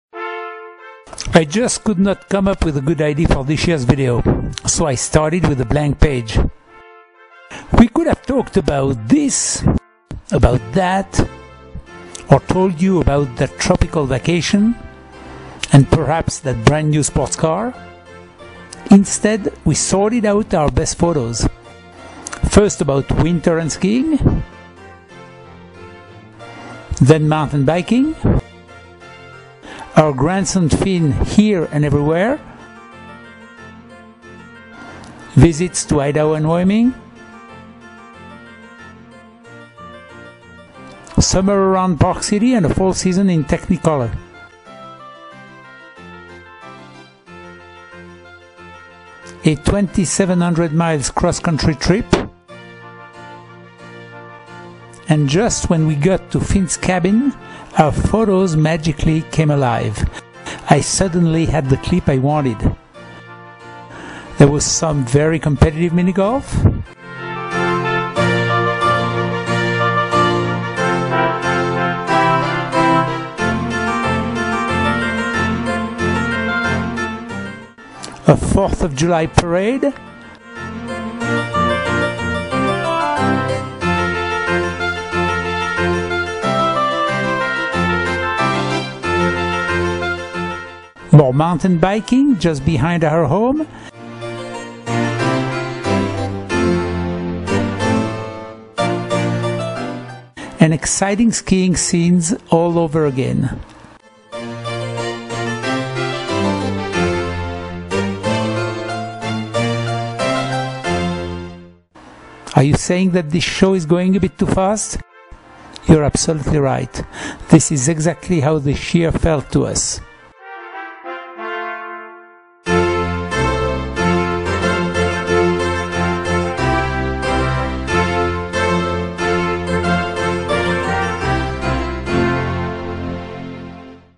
20121231-e
I just couldn't come up with a good idea for this year's video, so I started with a blank page...
We could have talked about this, about that...
Or told you about that tropical vacation...
and perhaps that brand new sports car...
Instead, we sorted out our best photos...
First about winter and skiing...
Then mountain biking...
Our grandson Finn here and everywhere...
Visits to Idaho and Wyoming...
Summer around Park City and a Fall season in technicolor...
A 4400 miles cross-country trip...
And just when we got to Finn's cabin, our photos magically came alive
I suddenly had the clip I wanted!
There was some very competitive mini-golf...
A 4th of July Parade...
More mountain biking just behind our home and exciting skiing scenes all-over again...
Are you saying that this show is going a bit too fast?
You're absolutely right! This is exactly how this year felt to us!